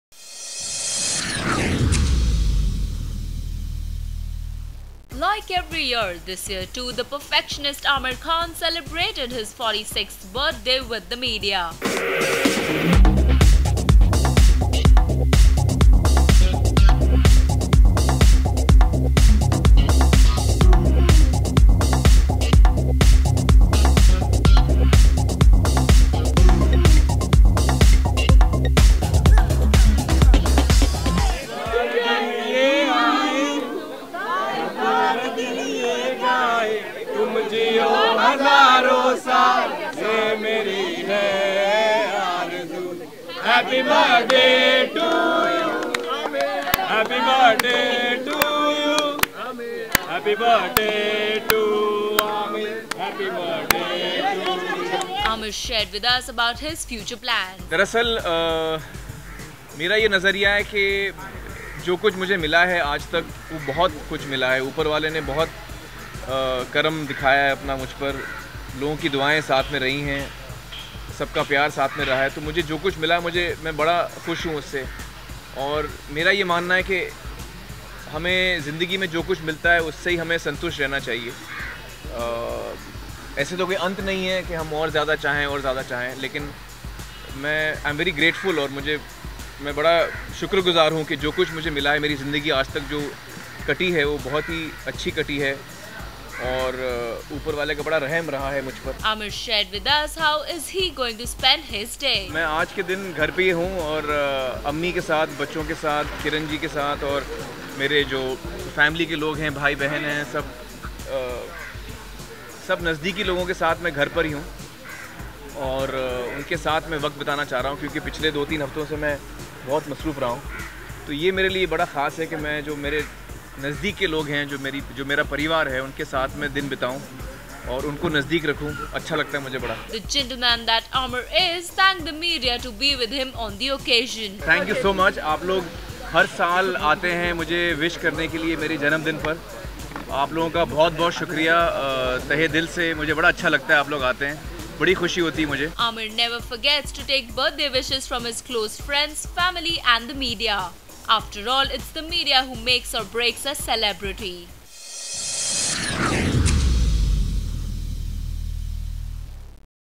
What's 'Aamir' doing for his 46th Birthday?
Watch Bollywood's most successful actor, the perfectionist 'Aamir Khan', as he celebrates his 46th Birthday with the media. Also catch the superstar talk about his birthday plans and his philosophies in life.